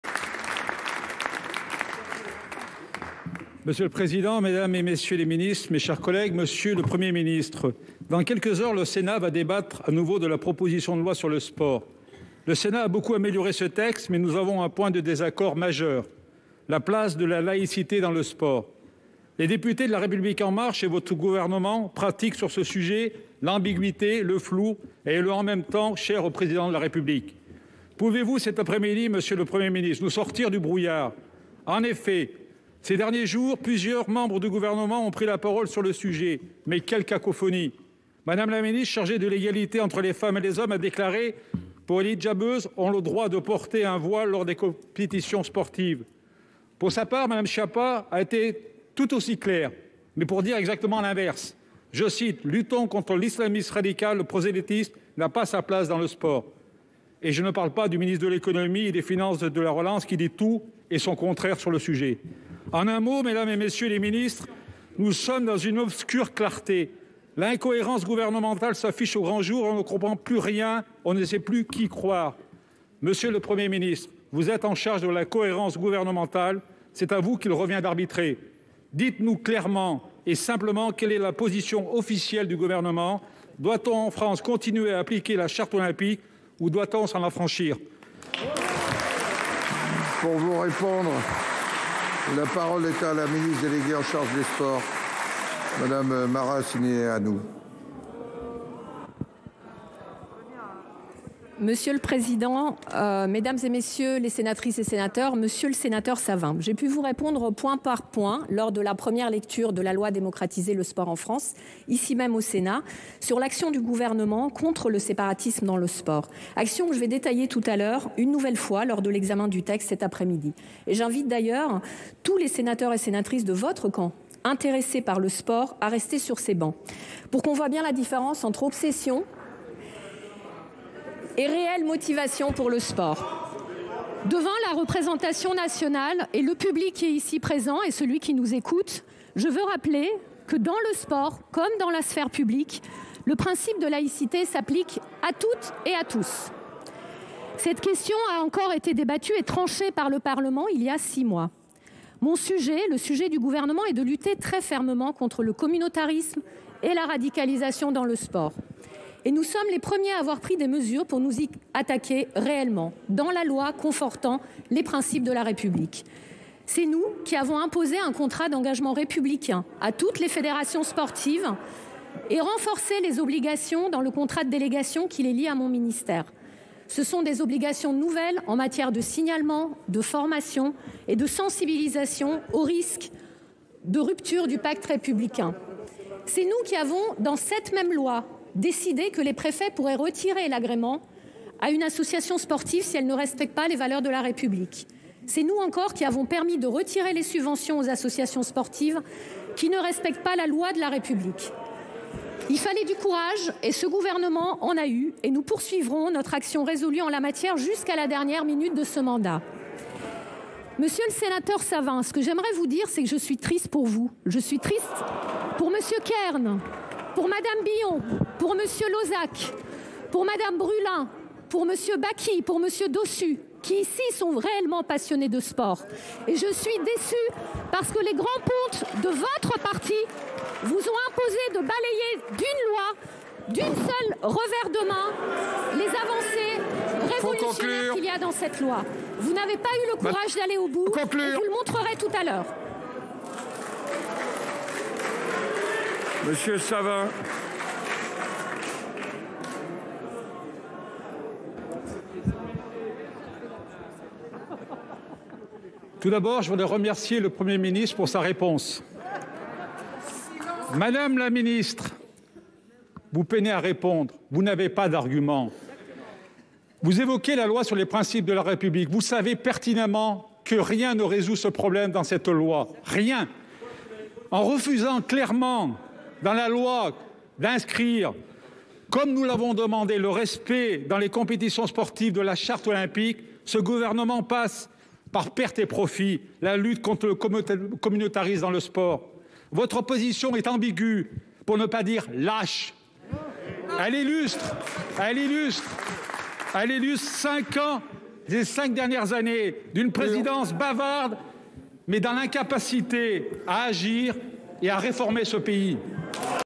Interdiction du voile dans le sport : Roxana Maracineanu dénonce « l’obsession » des sénateurs LR
Lors des questions d’actualité au gouvernement du Sénat, Michel Savin, a dénoncé « l’ambiguïté » du gouvernement sur une mesure portée par la majorité sénatoriale et qui vise à interdire le port de signes religieux dans les compétitions sportives.